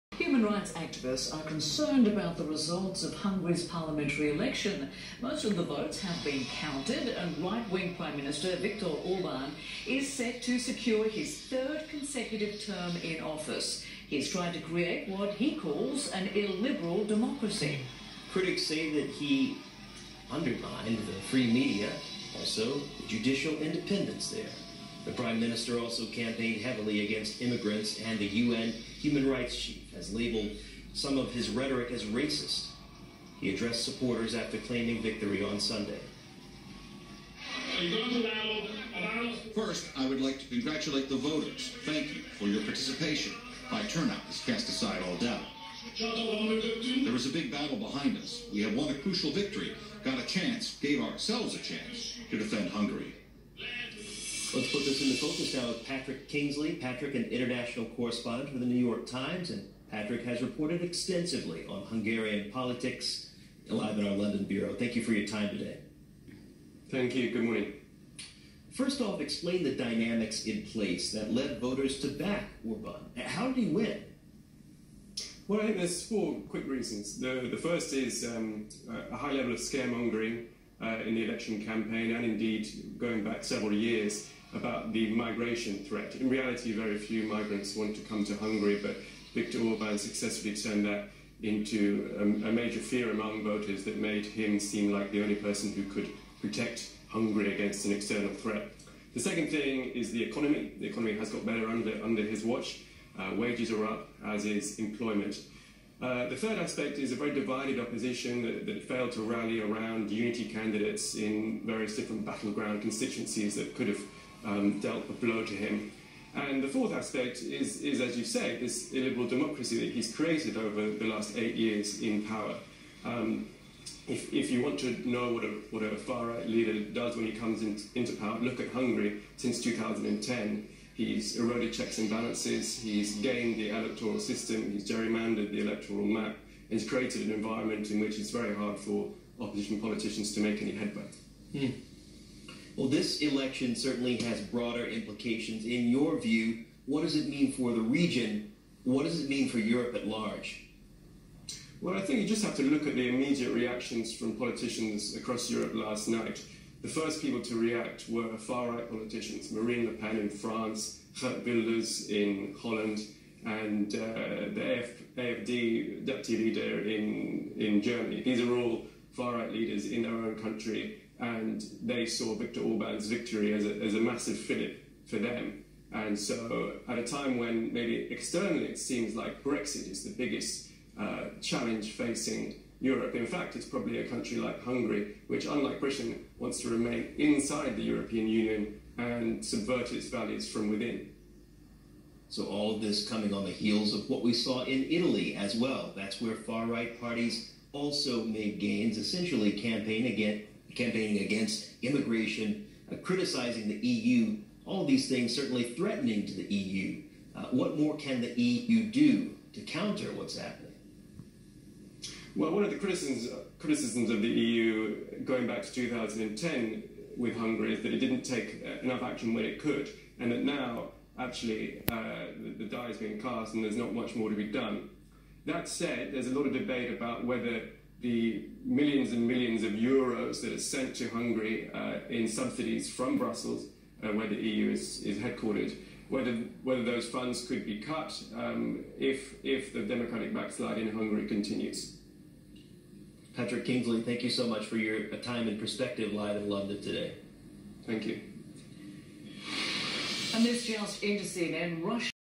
Patrick Kingsley on CNN talking about the 2018 Hungarian elections
Patrick Kingsley on CNN talking about Viktor Orban and the 2018 Hungarian elections.